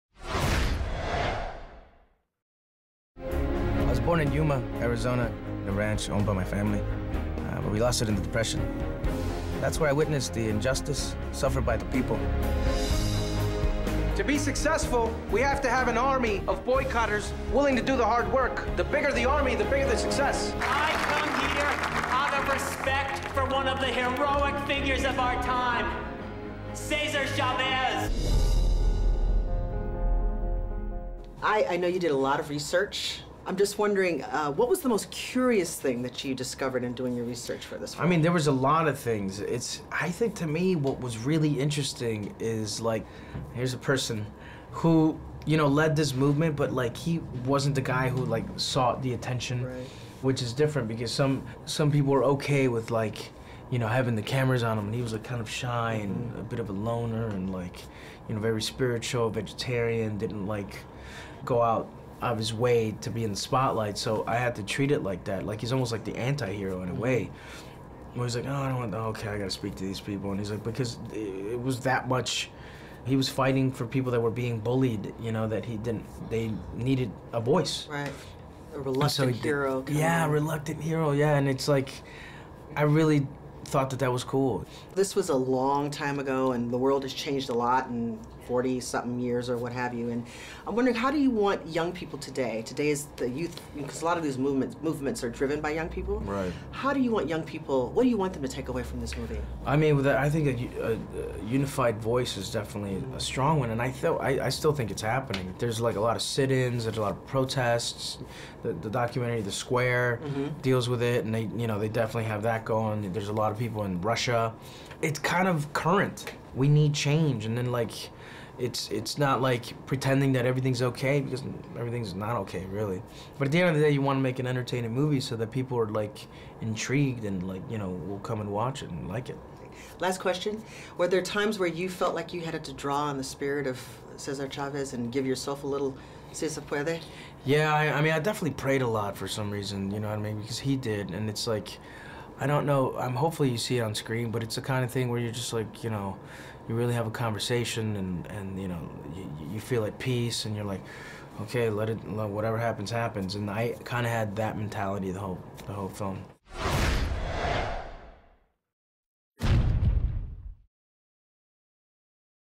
Interview with Michael Pena for Cesar Chavez - Just Seen It
Leah discussed the new biopic about the migrant farm worker leader with its star, Michael Pena.